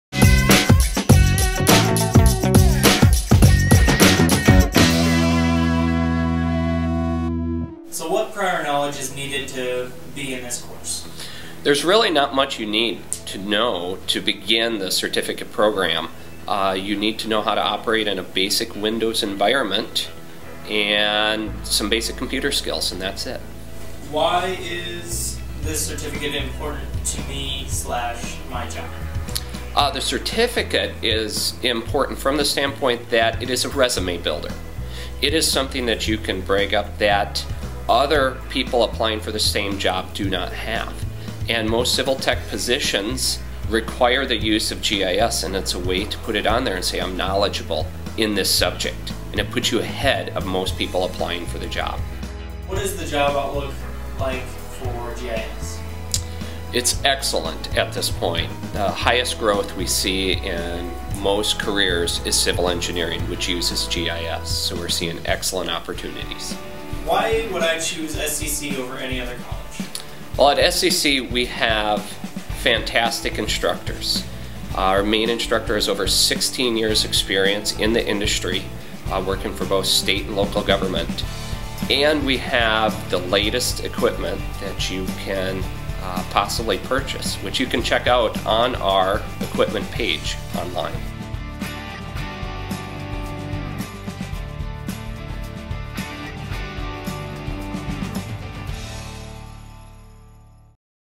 Geographic Information Systems at SCC
Why you might consider perusing a GIS certificate at our community and technical college in North Mankato, Minnesota.